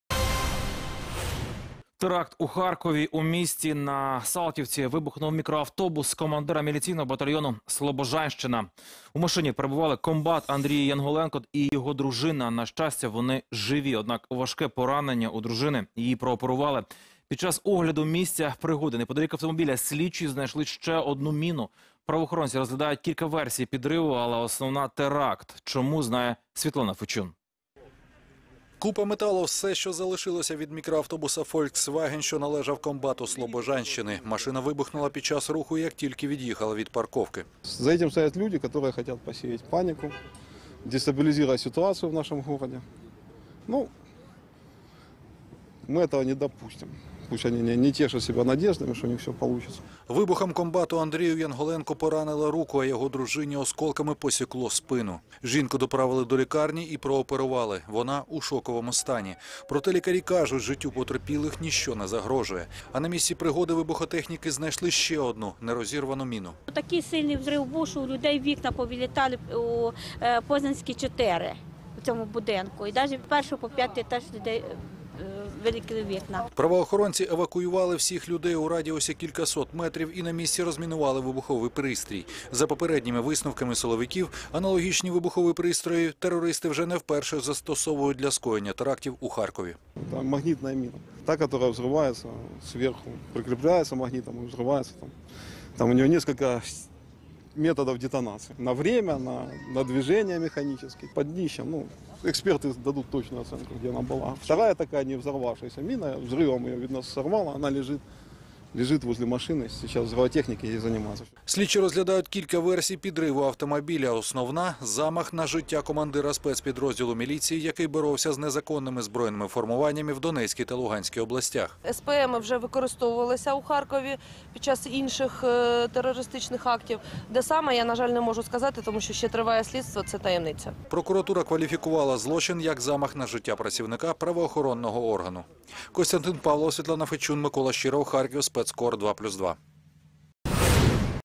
Теракт у Харкові
У місті на Салтівці вибухнув мікроавтобус командира міліційного батальйону Слобожанщина. У машині перебували комбат Андрій Янголенко і його дружина. На щастя, вони живі.